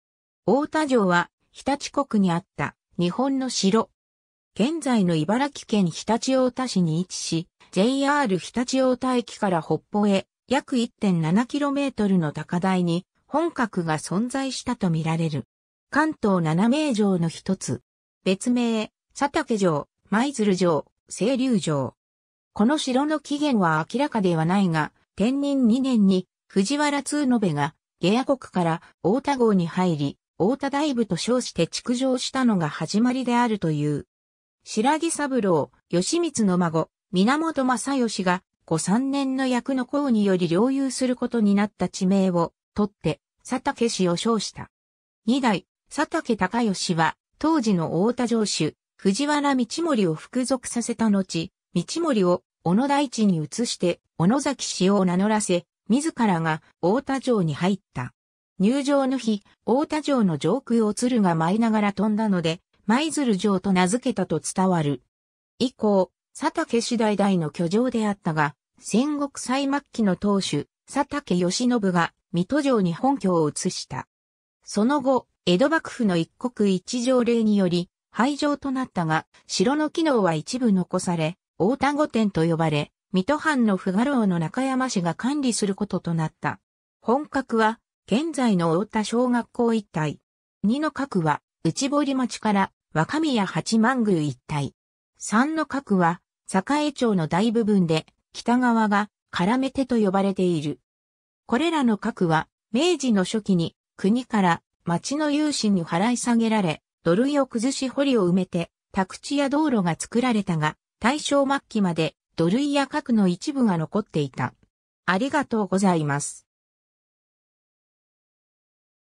太田城 (常陸国)
太田城（おおたじょう）は常陸国にあった日本の城。
現在の茨城県常陸太田市に位置し、JR常陸太田駅から北方へ約1.7kmの高台（現在の太田小学校）に本郭が存在したとみられる。
関東七名城の一つ。
別名：佐竹城、舞鶴城、青龍城。
 この城の起源は明らかではないが、天仁2年（1109年）に藤原通延（藤原秀郷の四世）が、下野国から太田郷に入り、太田大夫と称して築城したのが始まりであるという。
 新羅三郎義光の孫・源昌義が、後三年の役（1051年から1062年）の功により領有することになった地名をとって「佐竹氏」を称した。
二代佐竹隆義は当時の太田城主藤原通盛（通延の孫）を服属させたのち、通盛を小野台地（瑞竜中学校周辺）に移して「小野崎氏」を名乗らせ、自らが太田城に入った。
入城の日、太田城の上空を鶴が舞いながら飛んだので「舞鶴城」と名づけたと伝わる。
 以降、佐竹氏代々の居城であったが、戦国最末期の当主佐竹義宣が水戸城に本拠を移した。
 その後、江戸幕府（徳川家康）の一国一城令により廃城となったが、城の機能は一部残され「太田御殿」と呼ばれ、水戸藩の附家老の中山氏が管理することとなった。
 本郭は現在の太田小学校一帯。
 二の郭は内堀町から若宮八幡宮一帯。
 三の郭は栄町の大部分で北側が搦手とよばれている。
 これらの郭は明治の初期に国から町の有志に払い下げられ、土塁をくずし堀を埋めて宅地や道路が造られたが、大正末期まで土塁や郭の一部が残っていた。